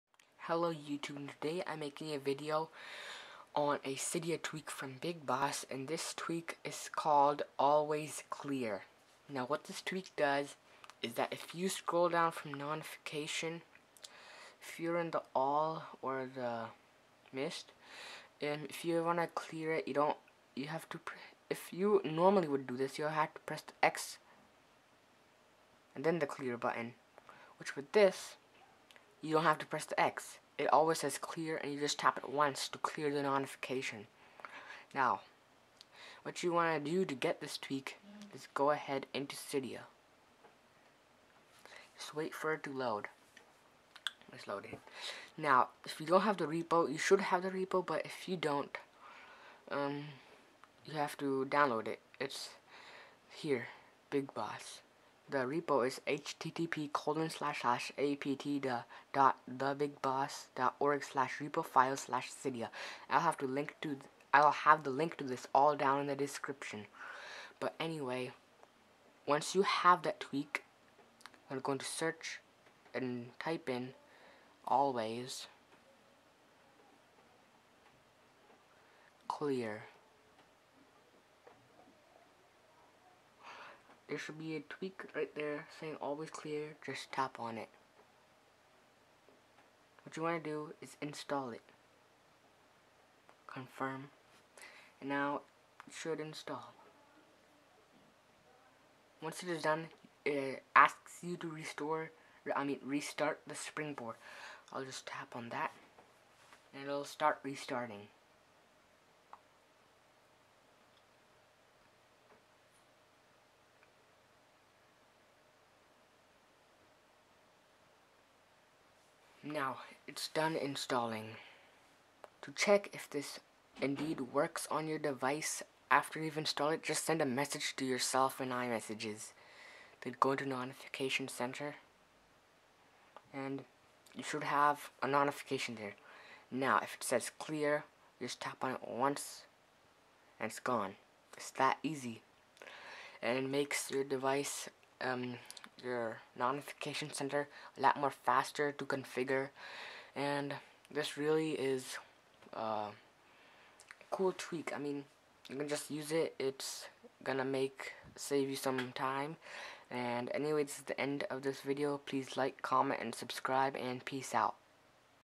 How to Clear notifications fast iOS 7 Jailbreak (Always Clear)
REQUIRMENTS:

Device running iOS 7

***THE REPO SHOULD BE BUILT IN SO YOU SHOULDN'T HAVE TO INSTALL IT***

This will make your device experience using notification center better. It make your device seem faster.